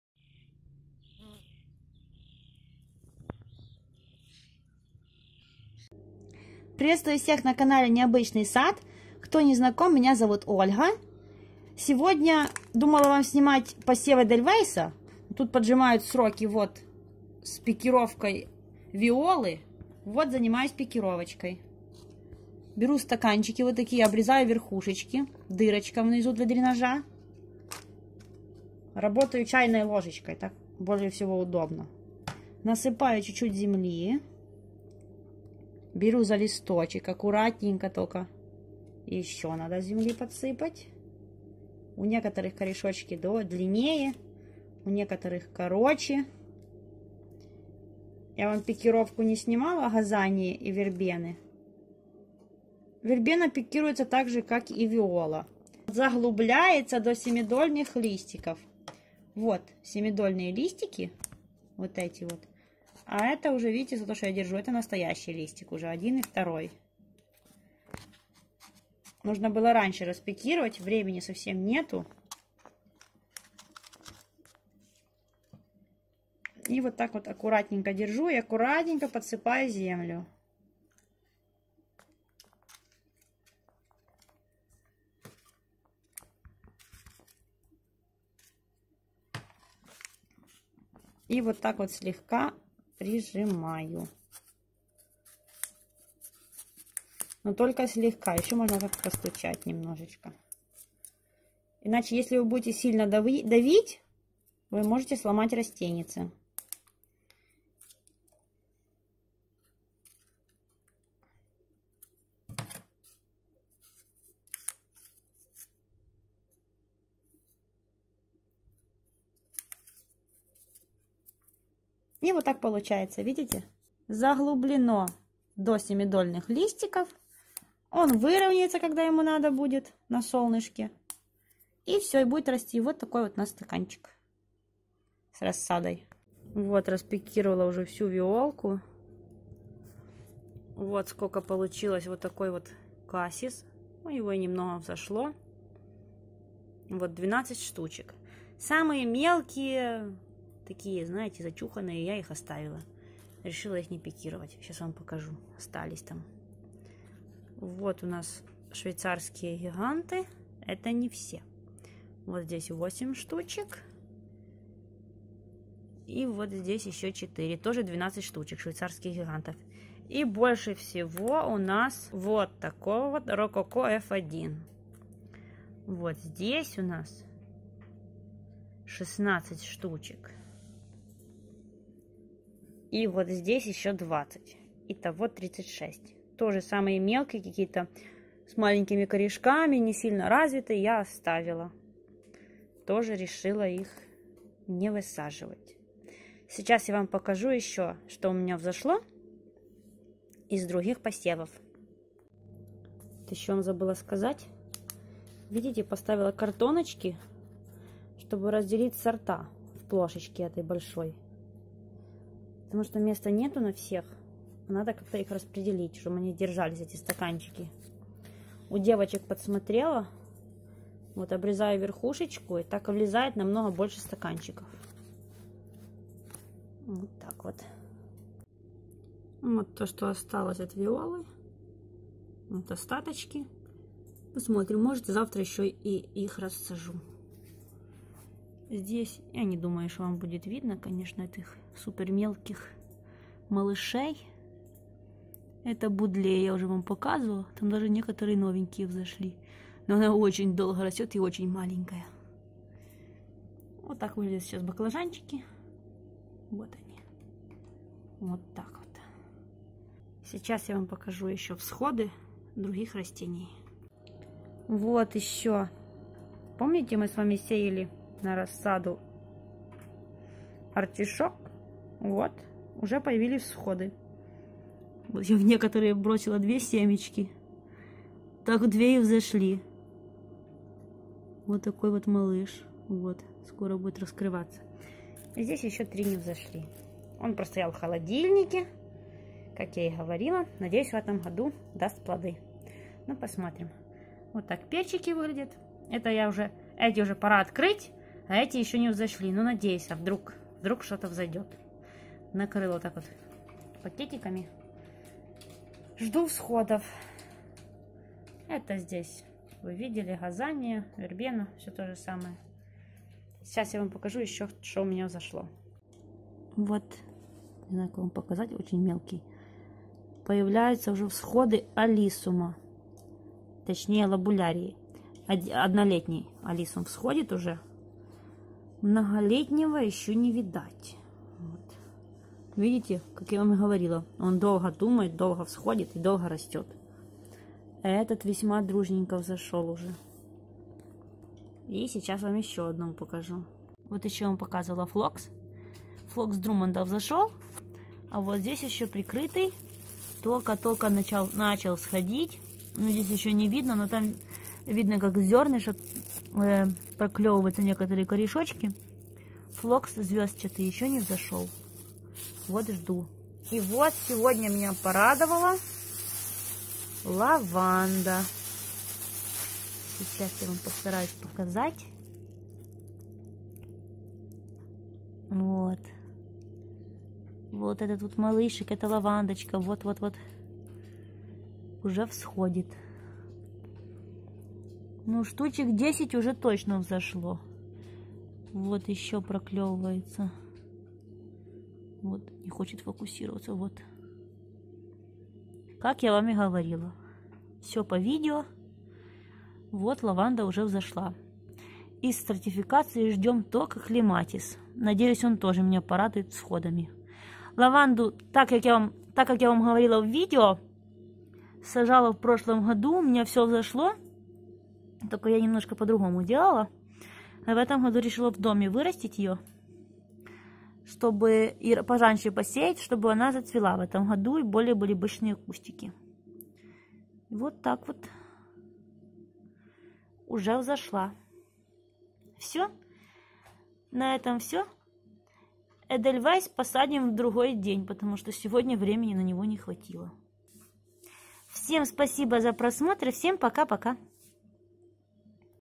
Пикировка рассады виолы /Всходы алиссума, лаванды, артишока 🌼🌸🌿
Решила показать вам как я пикирую виолу, и сколько рассады с трех пакетиков семян у меня вышло.
Также показываю вам результат посева лаванды, артишока и алиссума. И еще немного рассады которая подросла.
Если честно, то пикировка самое нелюбимое мое занятие при работе с рассадой. Для меня она отнимает слишком много времени и сил. И места после пикировки еще больше не хватает, ведь количество горшочков с одного увеличивается на десятки. Но никуда от этого не деться, без пикировки рассада будет плохо расти. Но нужно учитывать что не все растения любят пикировку и пересадку.